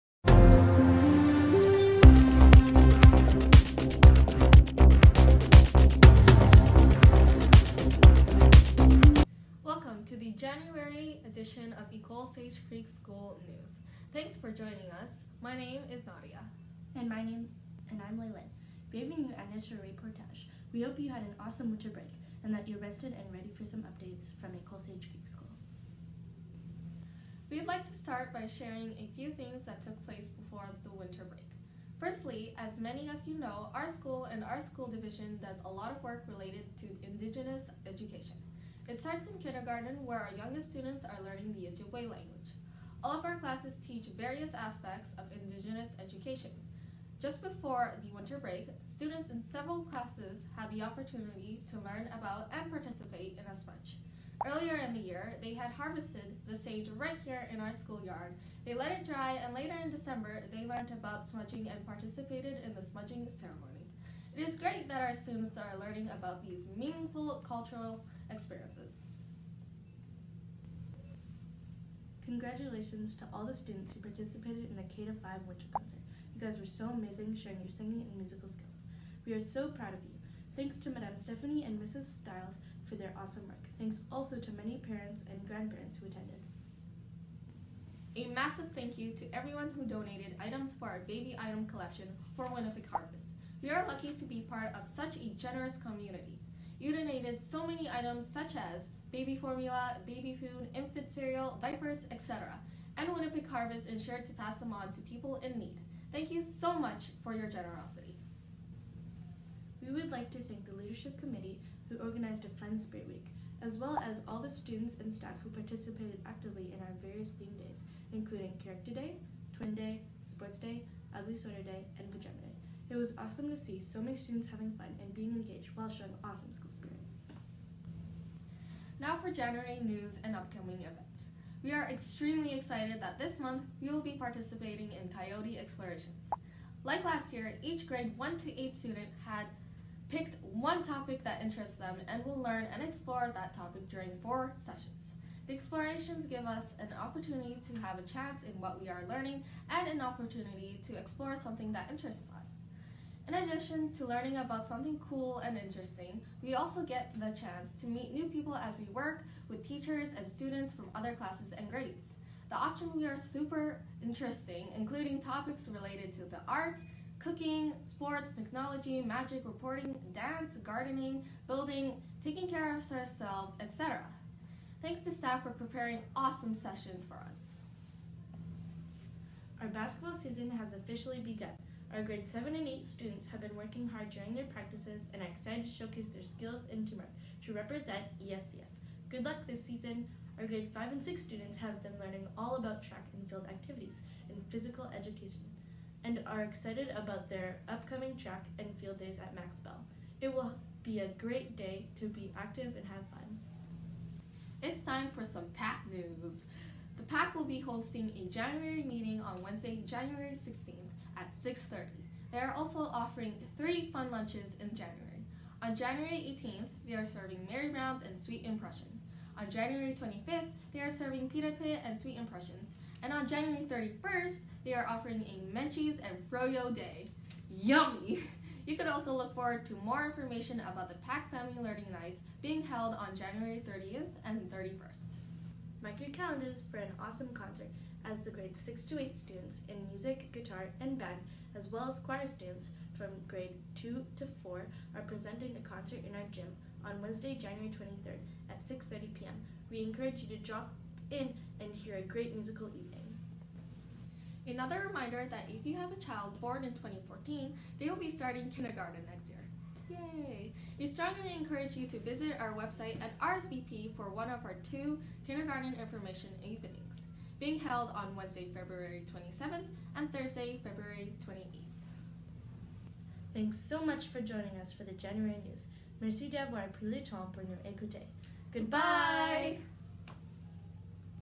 Jan 2019 ÉSCS News
Welcome back after our Winter Break.  Join our 2 newscasters who share various events that have taken place at ÉSCS as well as upcoming events.